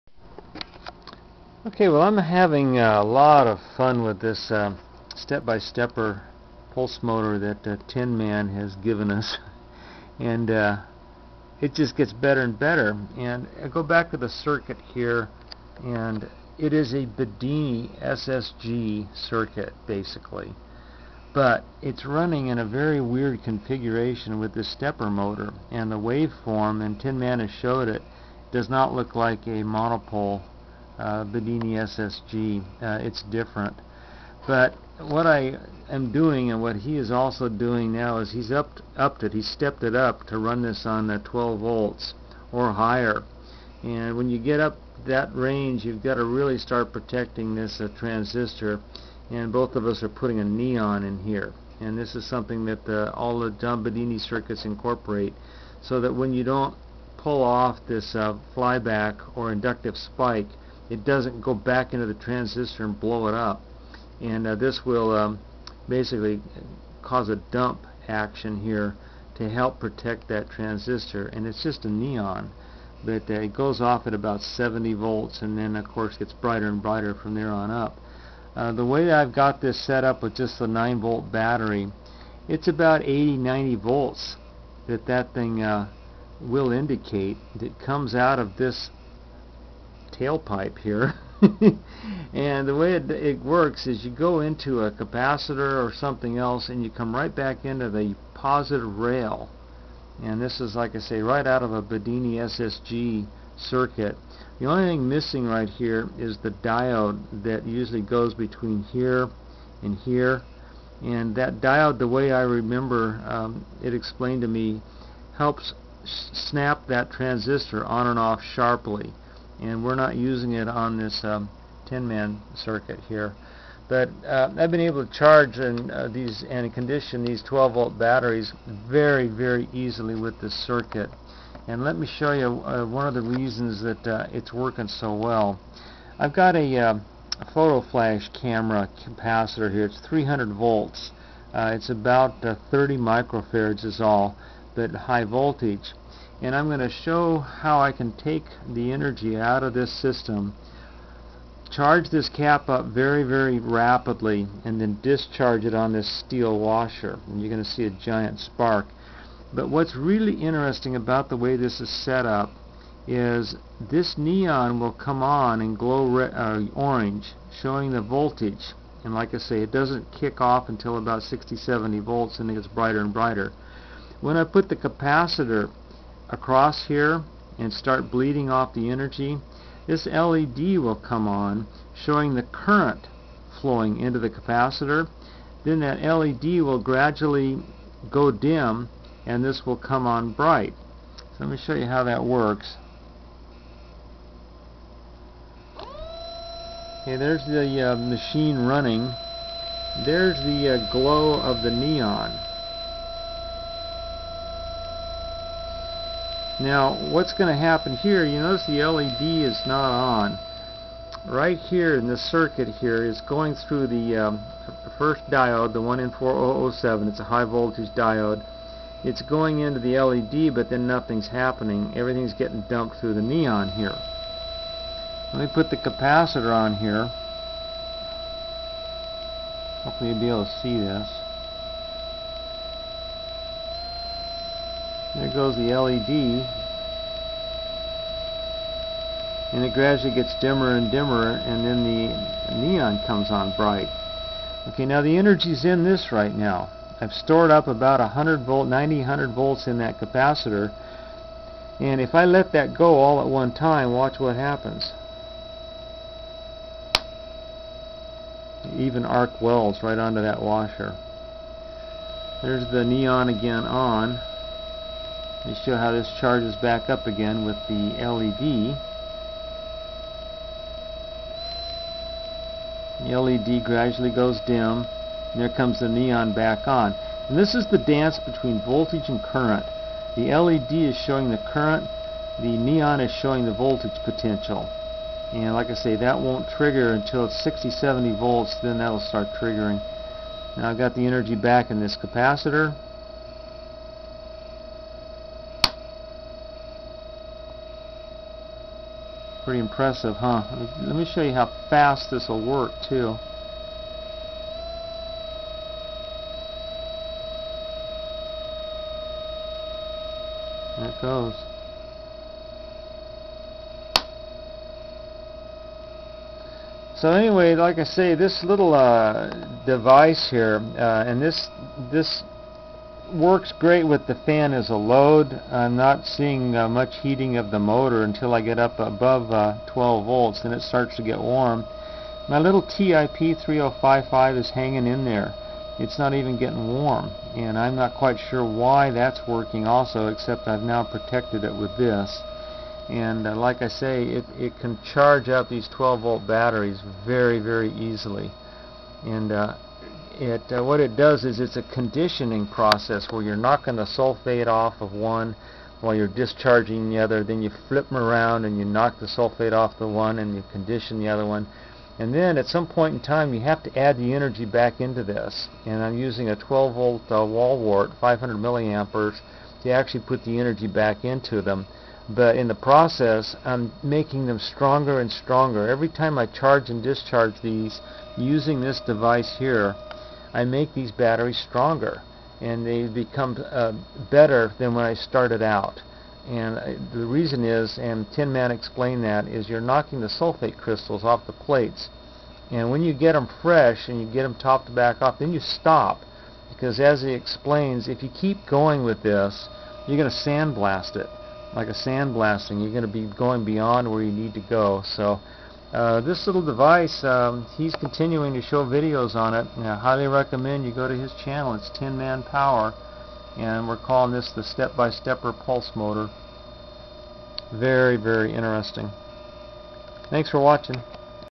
Step by Stepper --- neon protection
This is TinMan's 'Step by Stepper' pulse motor with a neon bulb added to the circuit to protect the transistor from high voltage kickback.